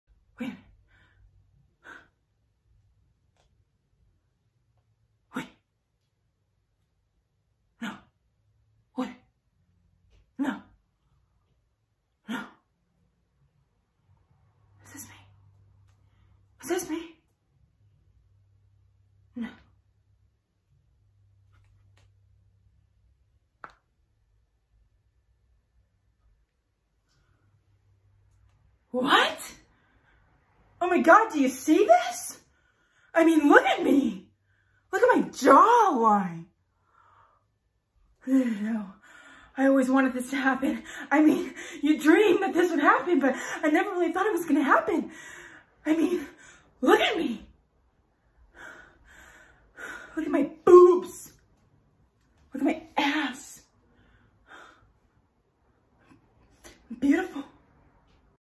-COMETIC MONOLOGUE
I have prepared a cometic monologue playing the character Rene Bennett from the movie “I Feel Pretty” Originally played by Amy Schumer